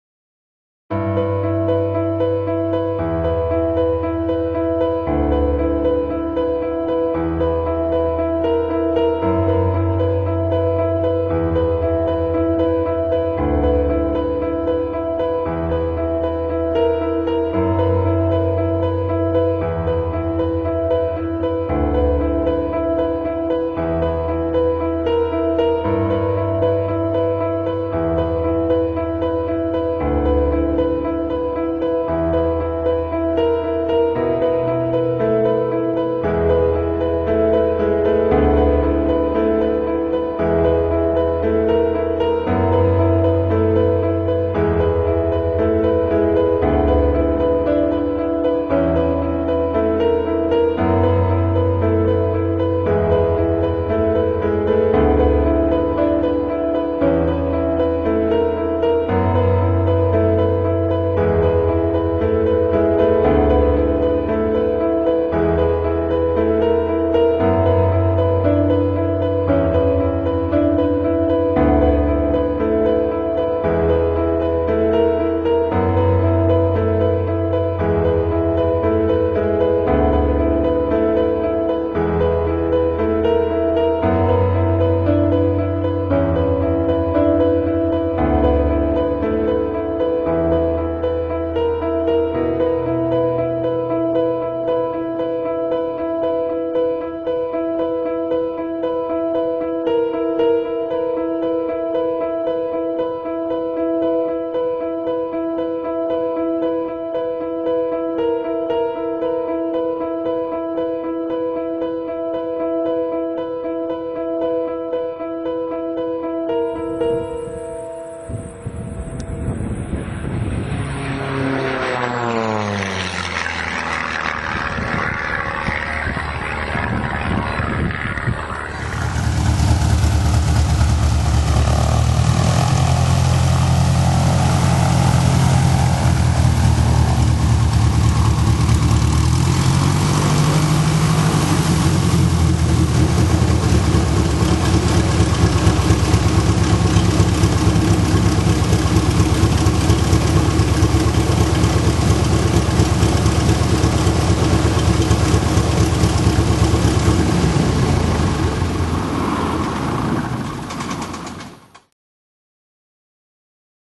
Unleashed Lancair Preparing for Reno Air Races 2010
Just a short flight formation flight around the area with a good friend.  We took some good pics of the plane at speed to make sure all the gear doors are tight.  See you at the Reno Air Races!